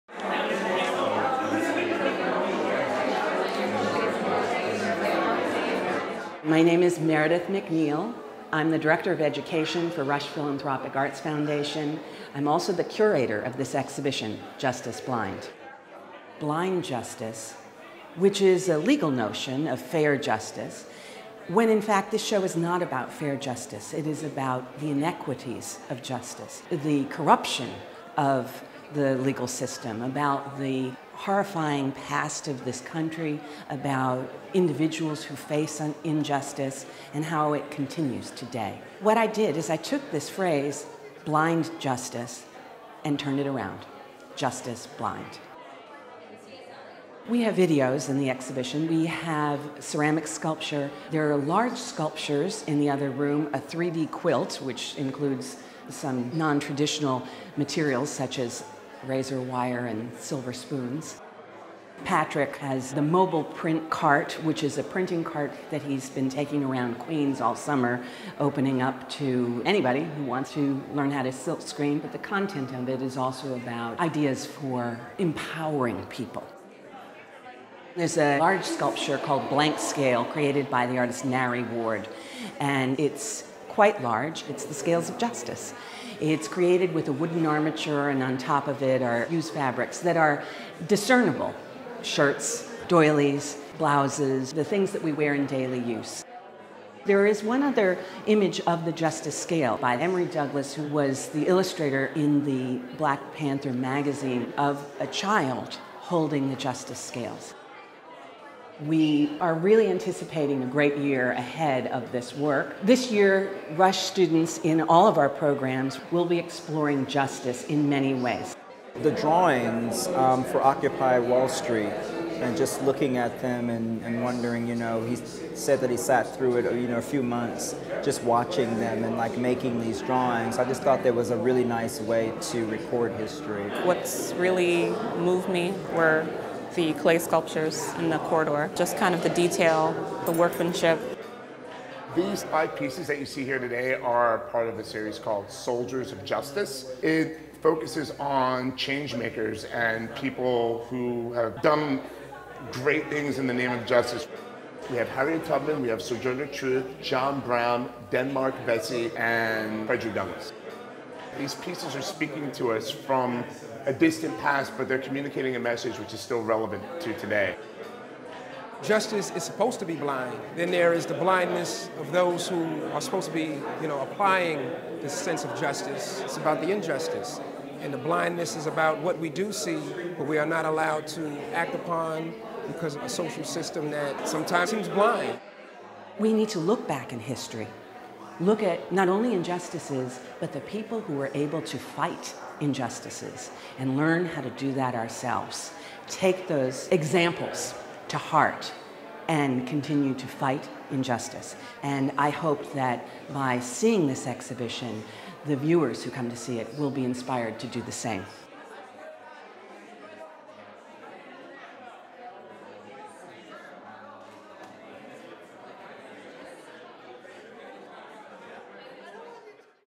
Justice Blind at Corridor Gallery Curated by Meridith McNeal with Rush Philanthropic Arts Foundation
JUSTICE BLIND
Corridor Gallery

Blind Justice is a legal concept that refers to the neutrality of the dispensing of justice. In naming this exhibition, I chose to reverse the word order of this phrase to reflect the dreadful inequities and miscarriages of justice that so many human beings grapple with in their everyday lives.
Working in a variety of mediums including sculpture, photography, printmaking, drawing, collage, video, interactive projects and performance, the artists in this exhibition question "justice" that is essentially blind to truth and, in their own ways, fight for freedom, fairness and equity.

Justice Blind will serve as a teaching tool for 2013/14 Rush Education programs. Kicking off our Year of Justice.

Millie Burns
Giuseppe Di Lelio
Emory Douglas
Gil Grimmett
Stephanie Lindquist
Miguel Luciano
Glendalys Medina
Vik Muniz
Lisa Peet
Patrick Rowe
Marie A. Roberts
Robert F. Skaff, Jr.
Pamela Talese
Nari Ward
Amanda Williams

Meridith McNeal, Curator